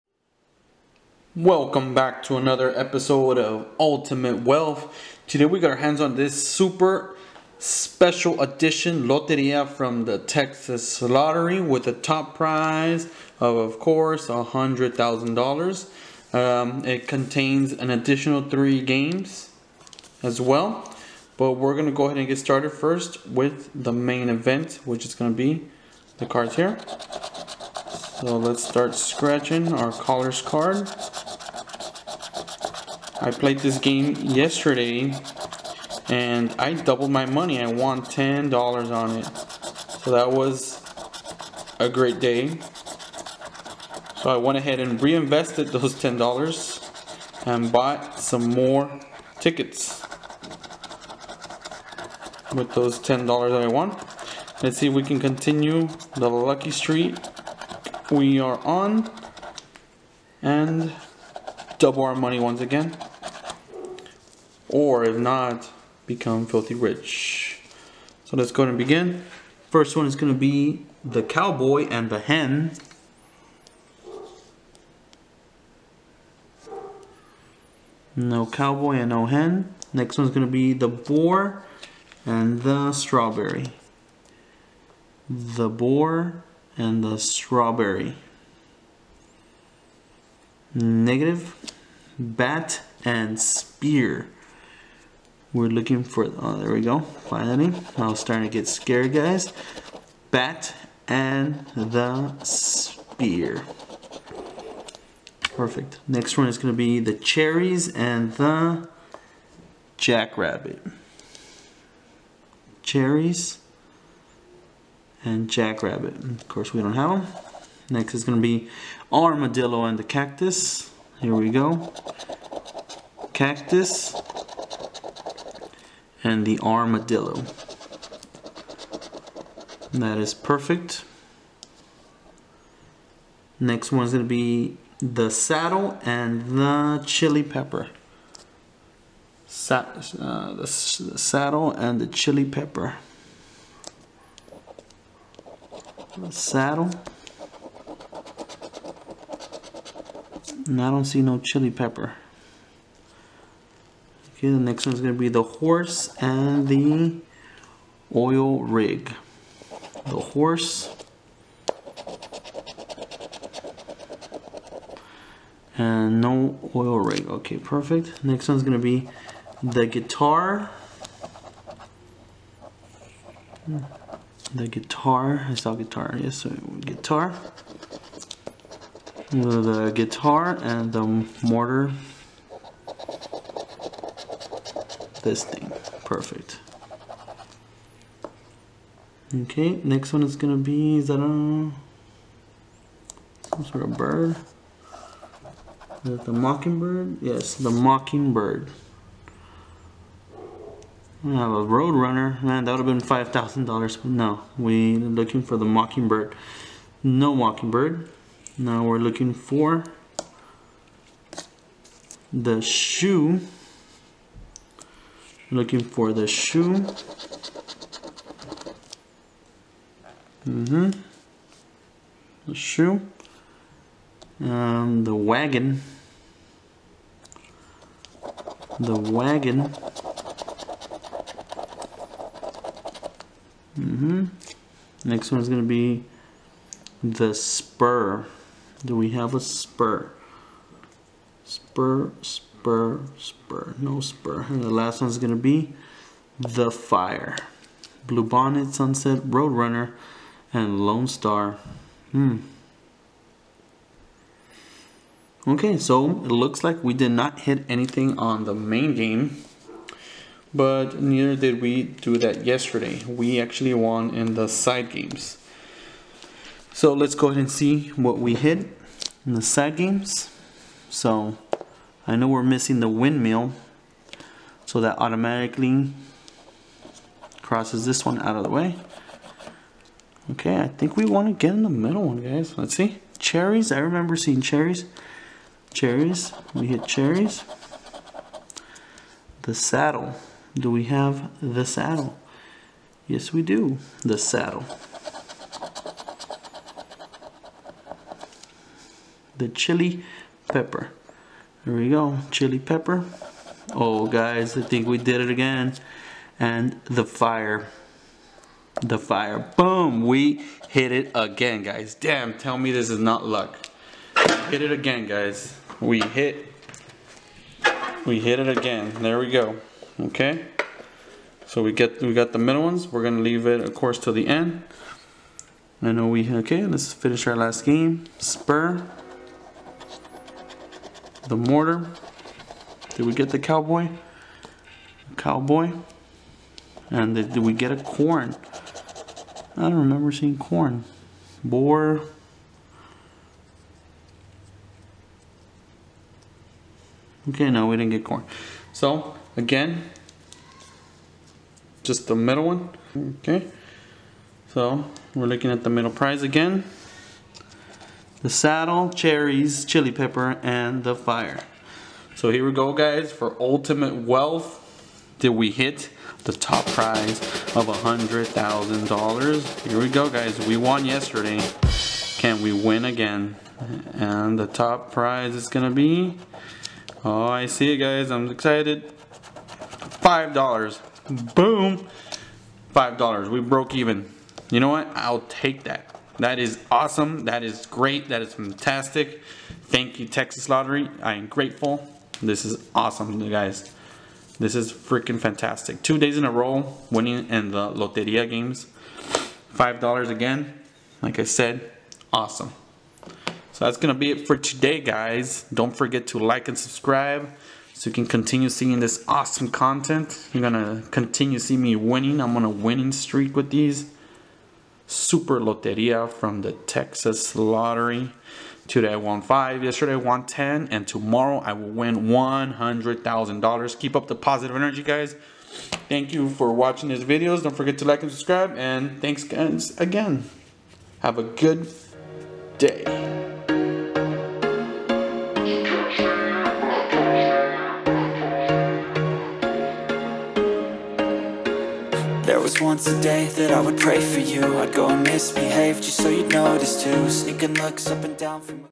Texaslottery |!!! I JUST KEEP WINNIG AT LOTERIA!!!
In this video, we dive into the exciting world of Loteria scratch off tickets! Loteria is a classic Mexican game of chance that has been enjoyed by many for generations. Now, you can experience the thrill of Loteria in the form of scratch off tickets.

In this ultimate challenge, I scratch off a variety of Loteria tickets to see if I can hit the jackpot. Each ticket has its own unique symbols and patterns, adding to the excitement of the game.

With every scratch off, the potential for a big win grows. From the iconic "El Corazon" to the "La Dama," every symbol is a chance to strike it rich.

Join me in this exhilarating journey as we uncover the secrets to winning big with Loteria scratch off tickets. Whether you're a seasoned player or a newcomer to the game, you won't want to miss this exciting challenge.

So grab your tickets, sit back, and get ready for an unforgettable adventure with Loteria scratch offs.